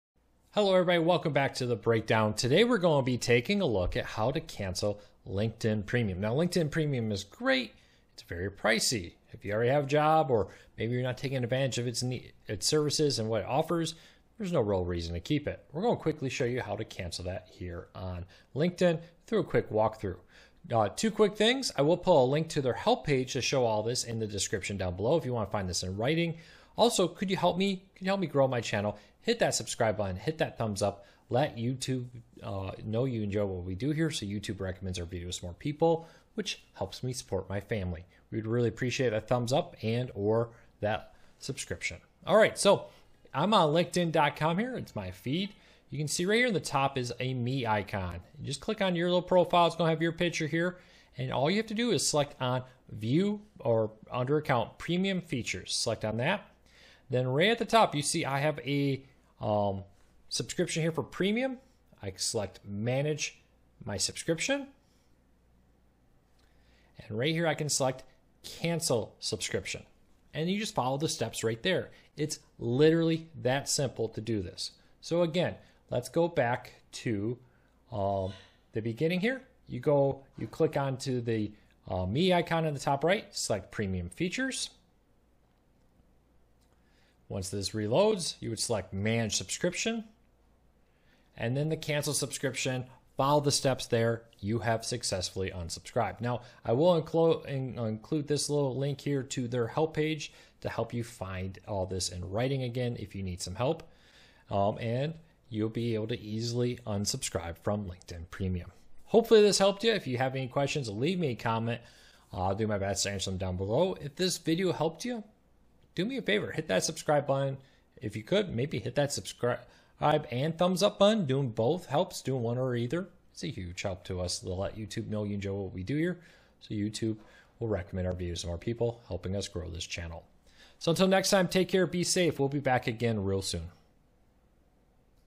How to Cancel LinkedIn Premium Subscription
Today we show you how to cancel LinkedIn Premium subscription. If you need more help you can find a step by step guide on the LinkedIn help